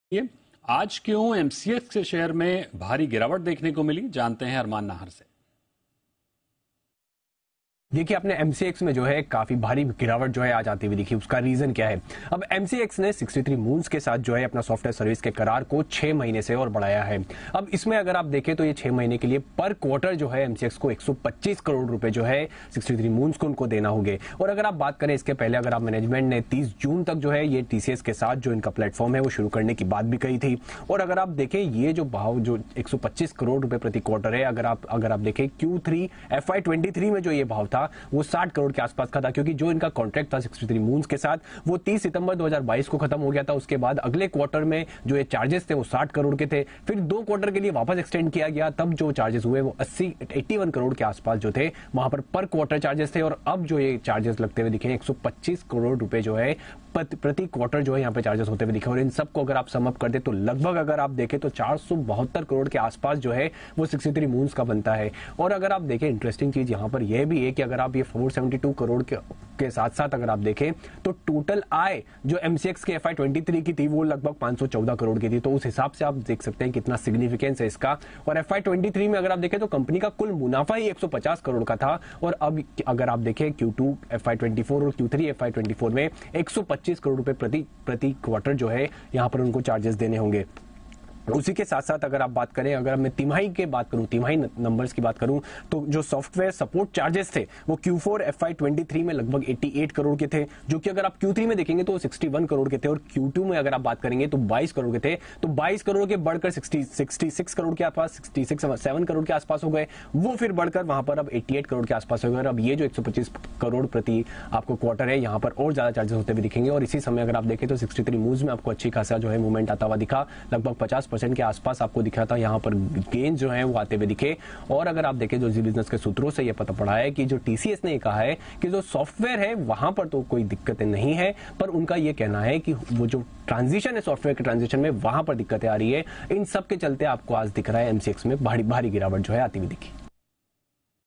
MCX Shares Witness Significant Decline, as it extends support services contract with 63 Moons
क्यों MCX के शेयर में भारी गिरावट?

63 MOONS के साथ करार क्यों बढ़ाया?

नए करार से MCX पर कैसा होगा असर?